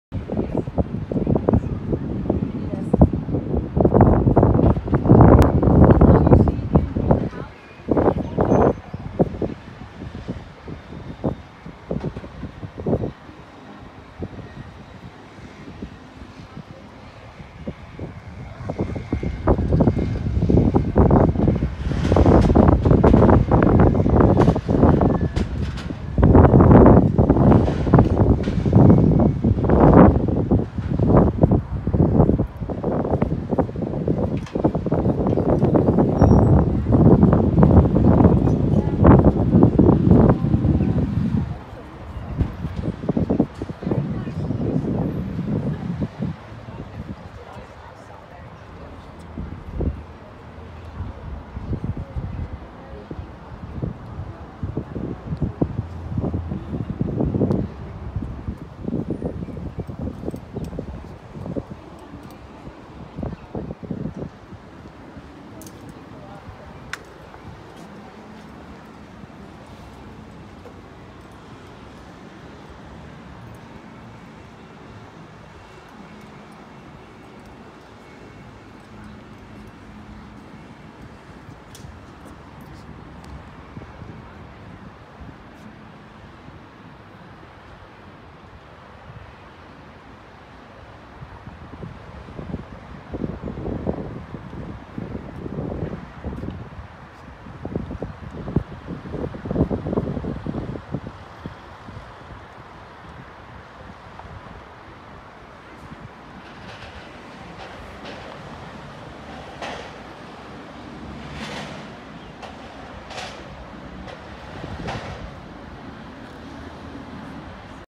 De Adriaan molen (windmill) in Haarlem - 2018-05-25
Viewing Haarlem from De Adriaan molen. 25 May 2018.